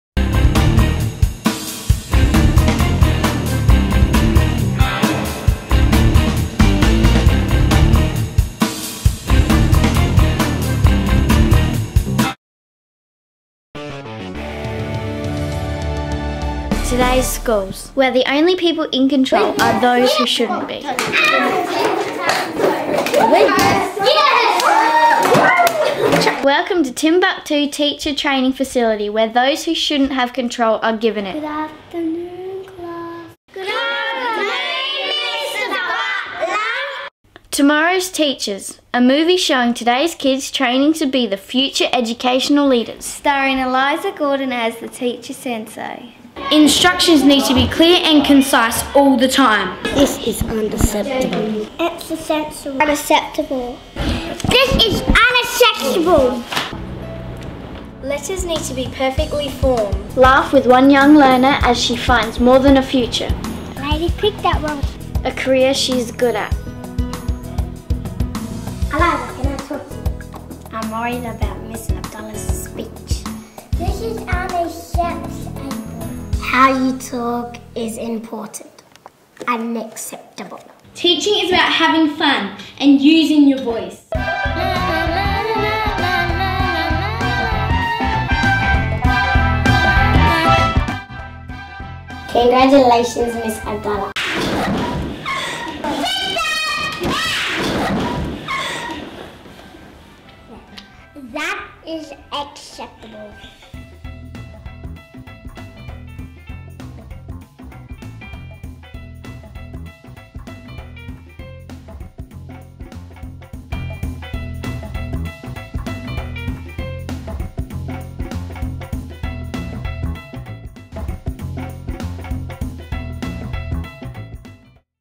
Make Me a Better Teacher
Winkie Primary School South Australia
Yr R-7

Our Movie Trailer is a humorous look at students training to be teachers. It highlights the the things we think will make teachers better to help with our learning.